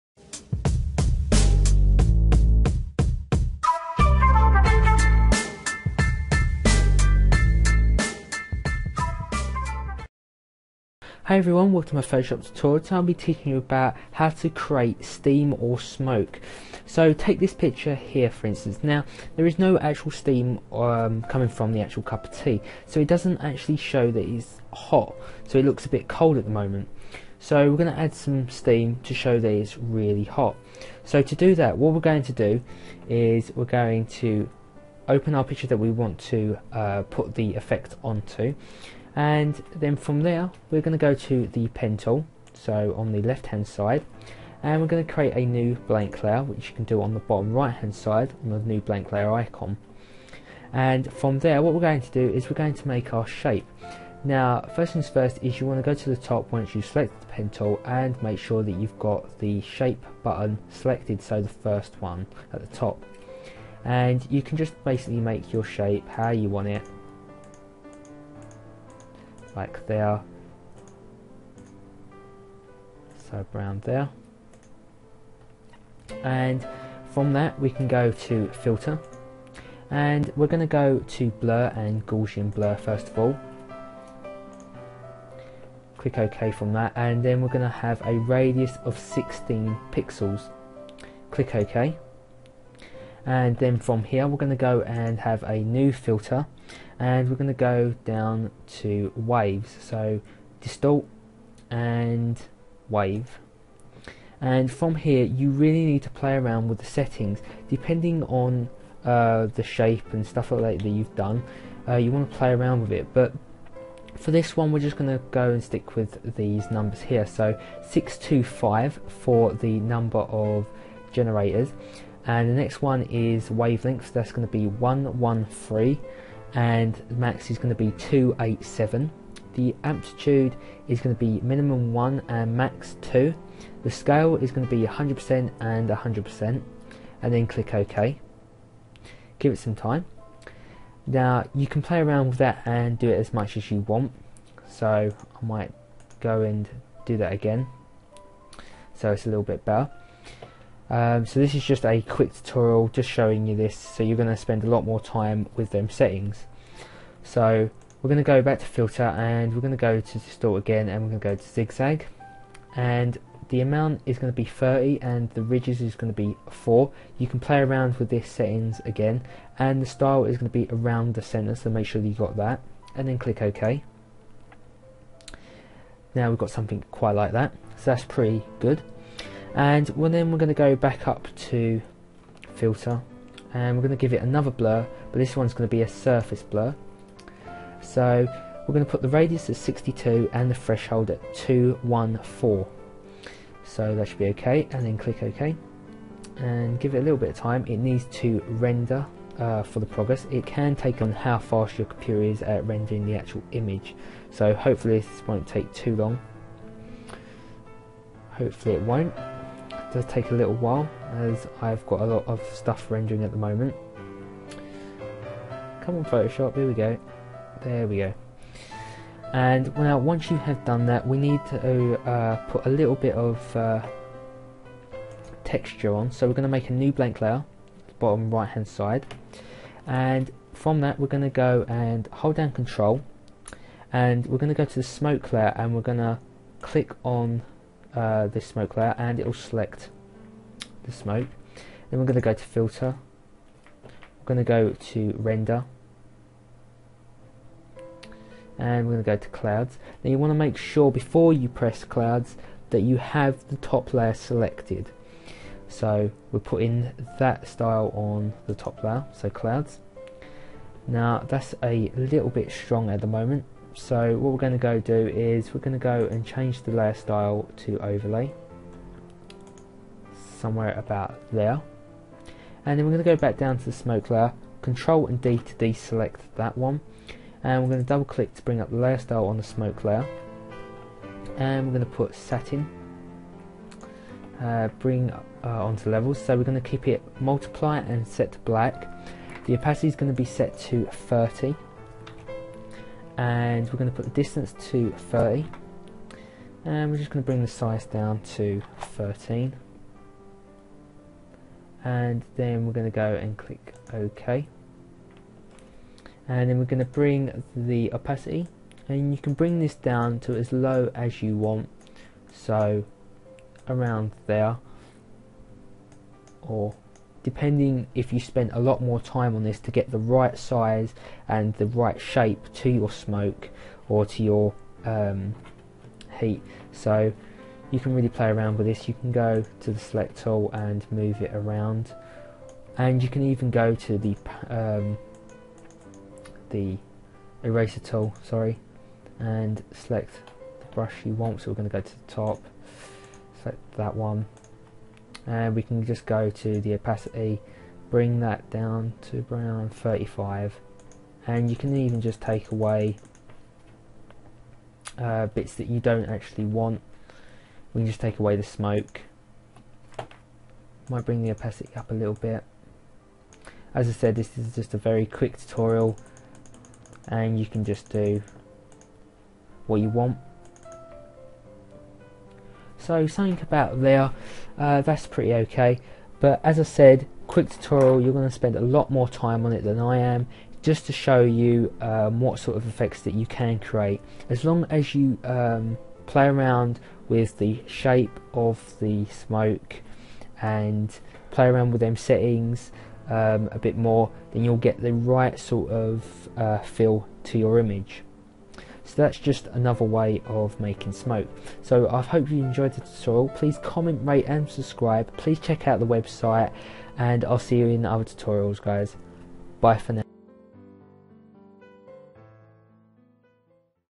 Photoshop CS5 Smoke/Steam Tutorial
This tutorial will teach you how to make smoke/steam in Photoshop so that you can apply this to most images so all your pictures can be perfect.

NEW T-SHIRTS!

Google Plus!